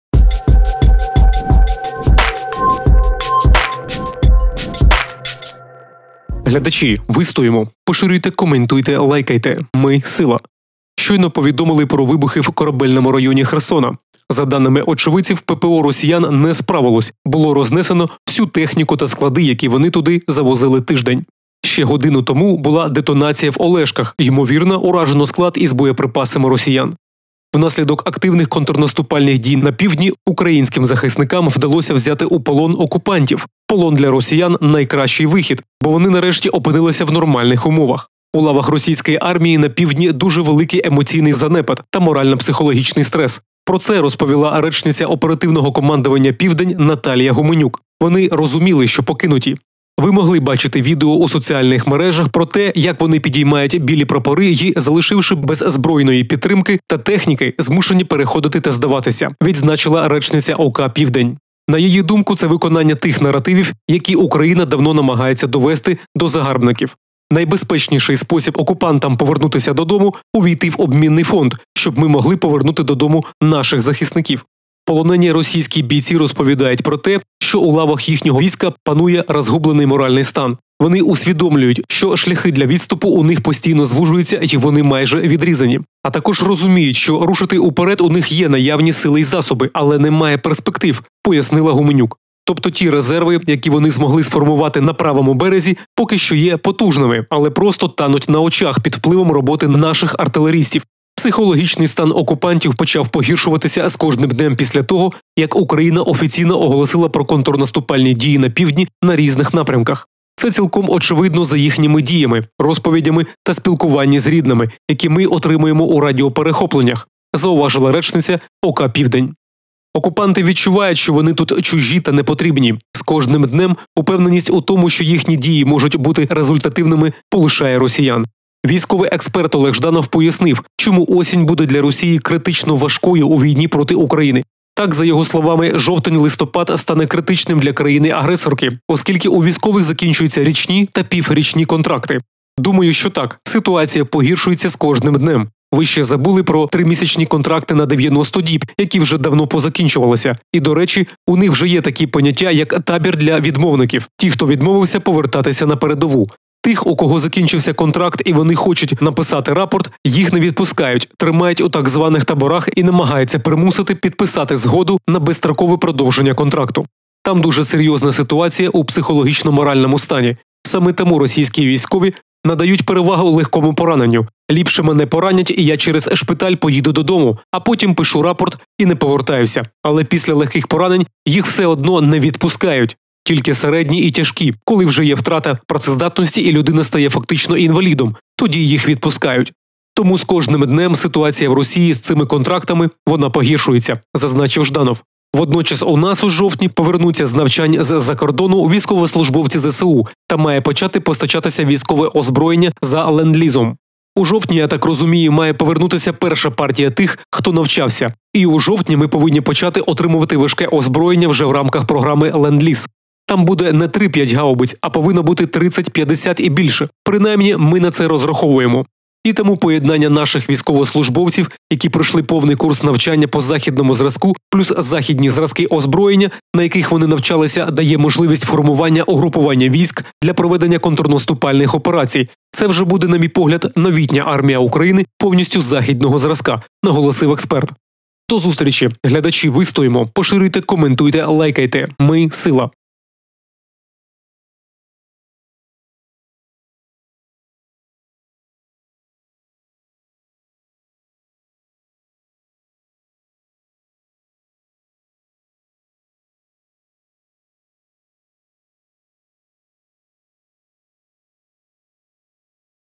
20 хвилин тому! Підняли руки - накрили в Херсоні. Бойові частини виводять з міста. Цілими ротами
Ось про що мовиться.